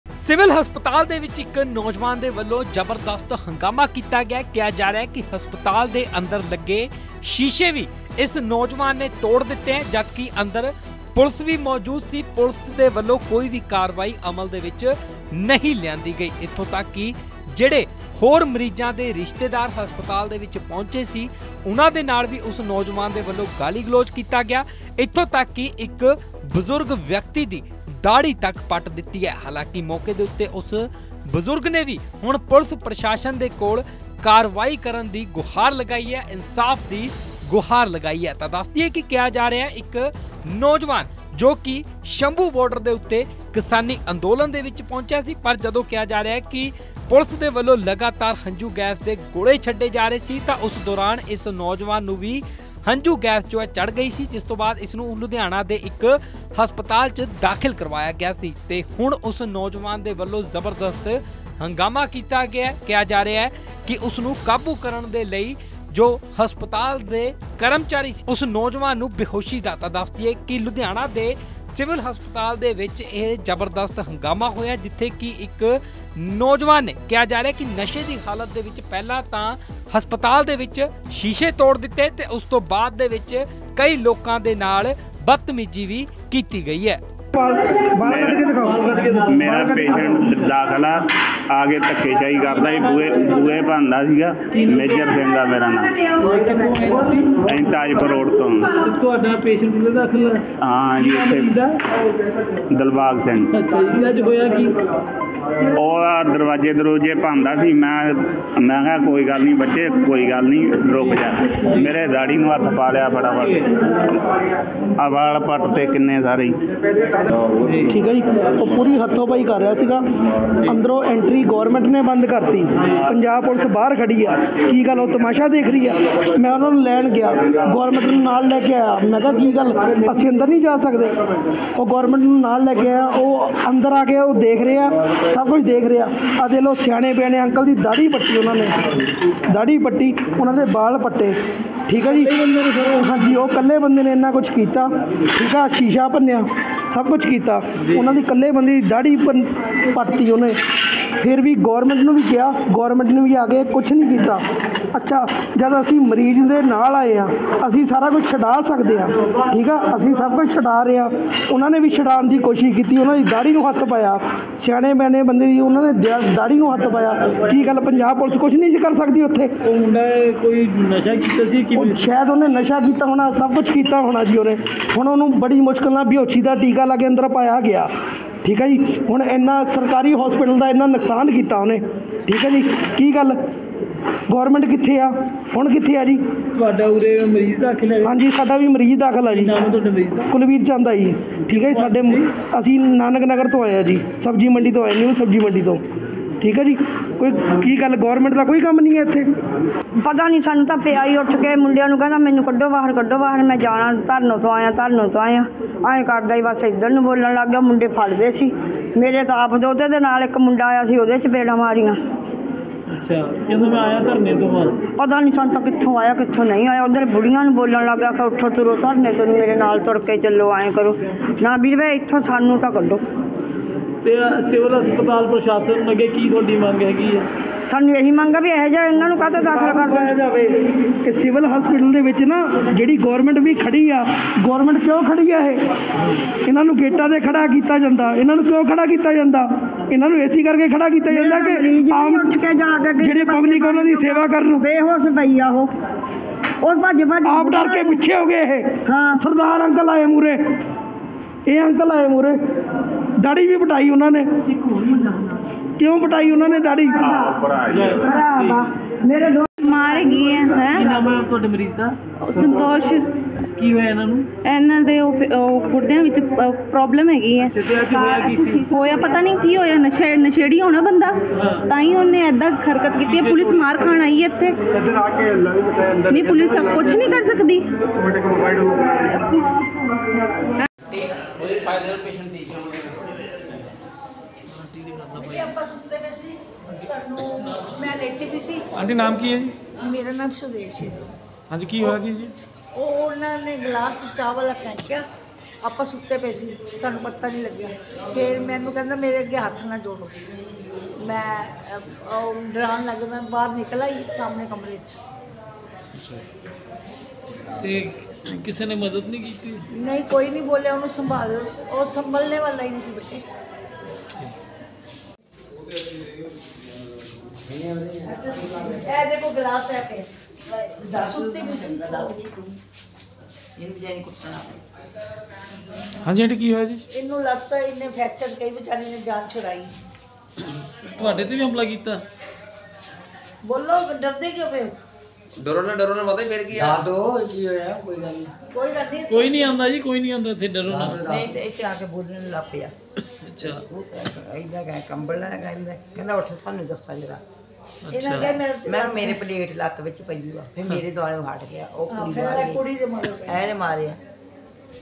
Ludhiana Clash | ਸ਼ੰਭੂ ਬਾਰਡਰ 'ਤੇ ਨੌਜਵਾਨ ਕਿਸਾਨ ਨੂੰ ਚੜ੍ਹੀ ਸੀ ਹੰਝੂ ਗੈਸ
ਲੁਧਿਆਣਾ ਦੇ ਸਿਵਲ ਹਸਪਤਾਲ ਵਿੱਚ ਦੇਰ ਰਾਤ ਇੱਕ ਨੌਜਵਾਨ ਨੇ ਹੰਗਾਮਾ ਮਚਾ ਦਿੱਤਾ। ਟਰਾਮਾ ਵਾਰਡ 'ਚ ਨੌਜਵਾਨ ਨੇ ਸ਼ੀਸ਼ਾ ਵੀ ਤੋੜ ਦਿੱਤਾ। ਦੱਸ ਦਈਏ ਕਿ ਨੌਜਵਾਨਾਂ ਨੇ ਵਾਰਡ 'ਚ ਦਾਖਲ ਮਰੀਜ਼ਾਂ 'ਤੇ ਹਮਲਾ ਕਰ ਦਿੱਤਾ। ਨੌਜਵਾਨ ਨੇ ਉਸ ਨੂੰ ਮਿਲਣ ਆਏ ਰਿਸ਼ਤੇਦਾਰਾਂ ਦੀ ਦਾੜ੍ਹੀ ਤੱਕ ਵੀ ਪੁਟੀ।